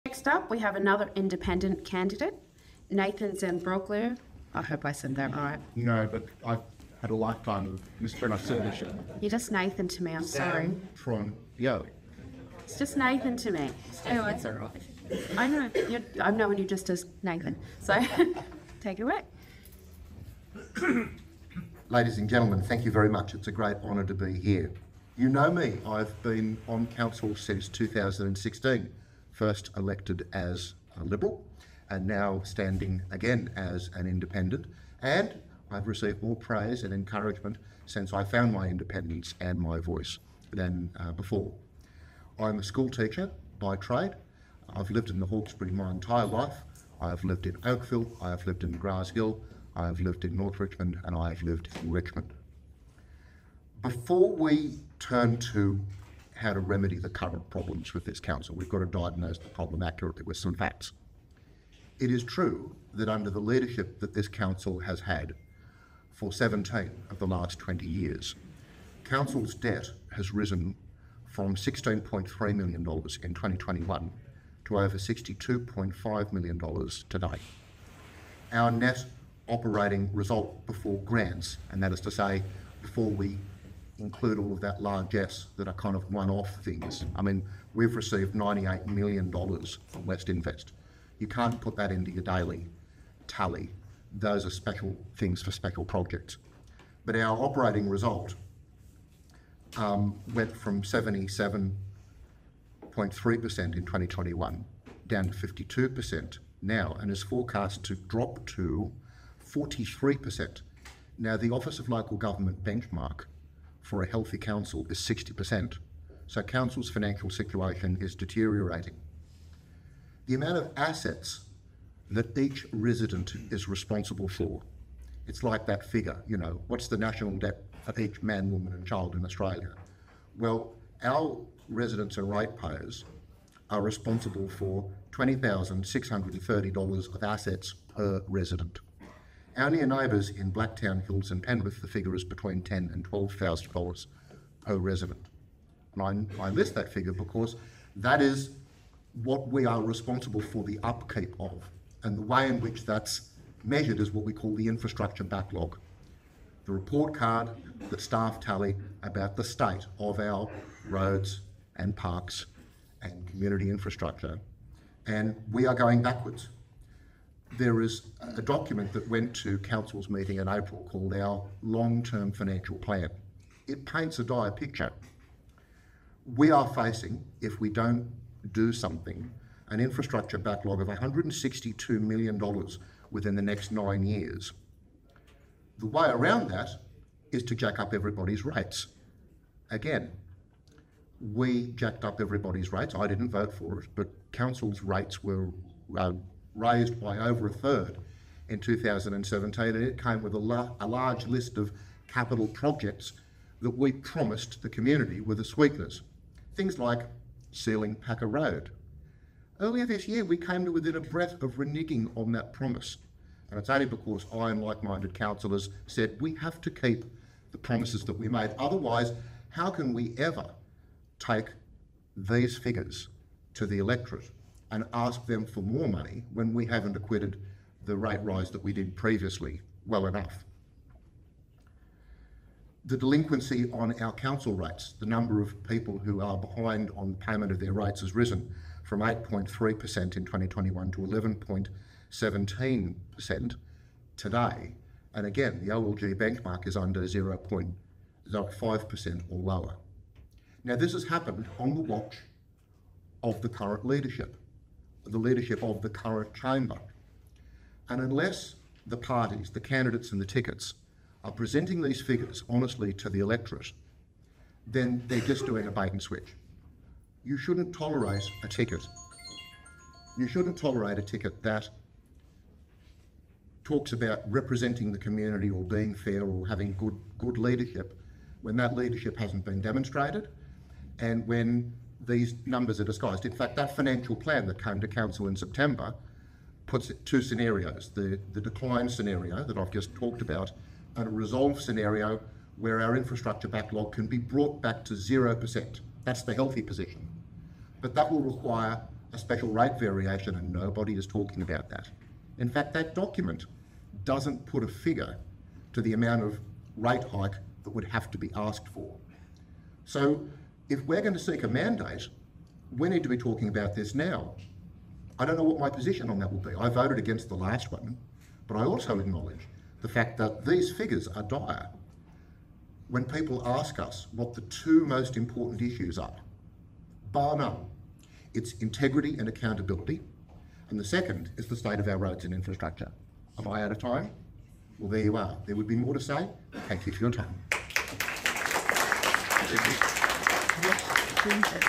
Meet the Candidates function hosted by the Windsor Business Group.
Today I joined some of the other ticket leaders at a 'meet the candidates' function hosted by the Windsor Business Group - WBG at the Windsor Bowling & Sports Club.

In the time I was given, I laid out some of the sobering statistics about what the next elected Chamber will be left to clean up after years of poor focus and a lack of leadership.

Thanks also to Stacey at the Prismatic Group, Kathryn Gene of Pulse 89.9FM Radio  and Hawkesbury Hills Catering Company.